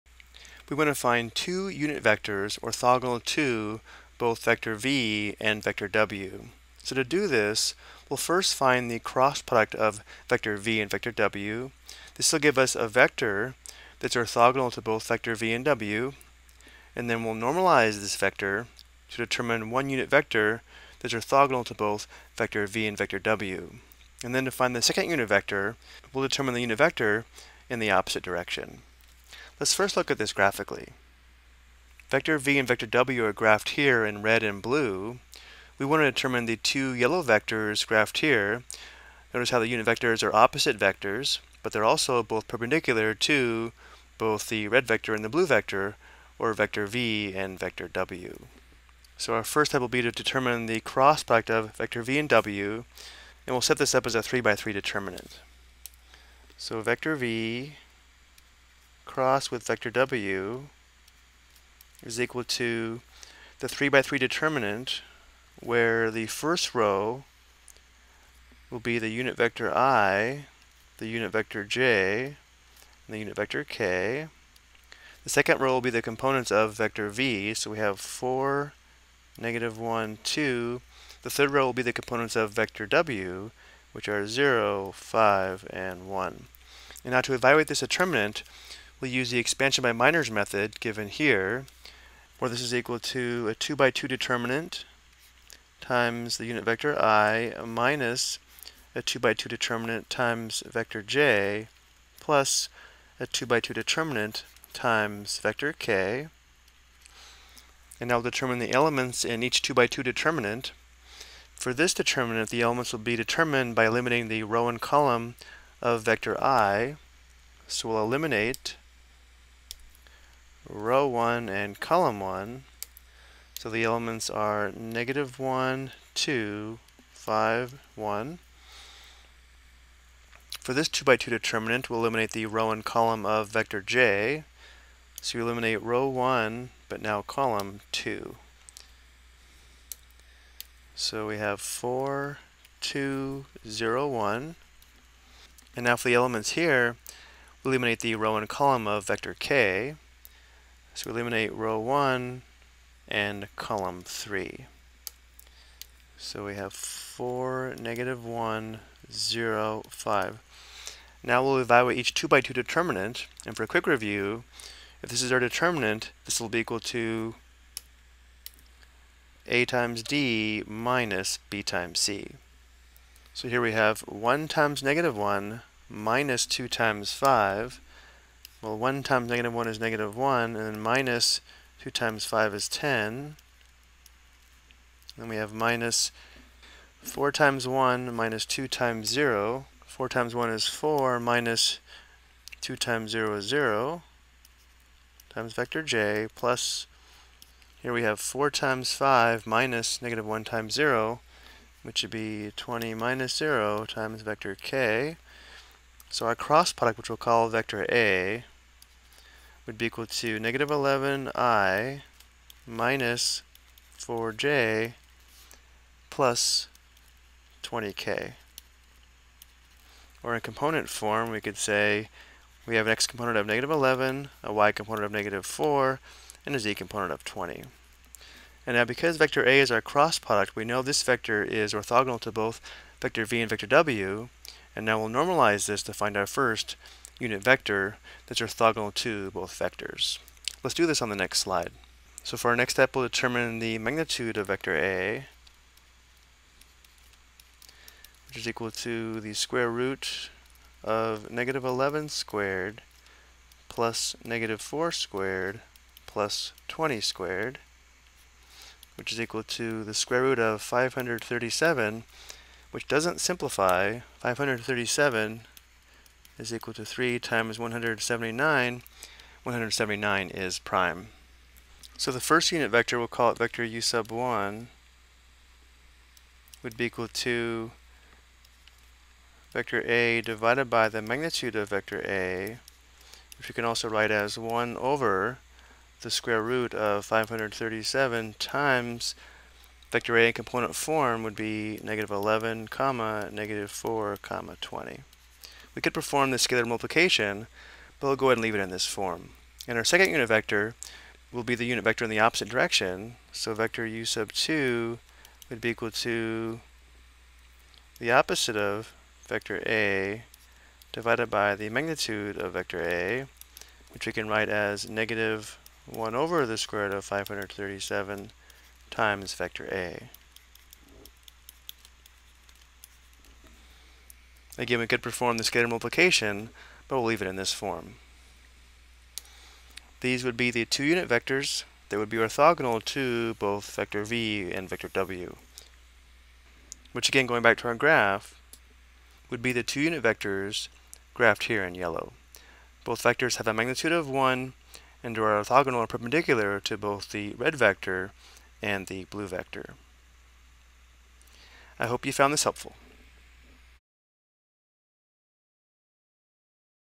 Ex: Find Two Unit Vectors Orthogonal to Two Given Vectors
This video explains how to use the cross product of two vectors to determine two unit vectors that are orthogonal to two given vectors.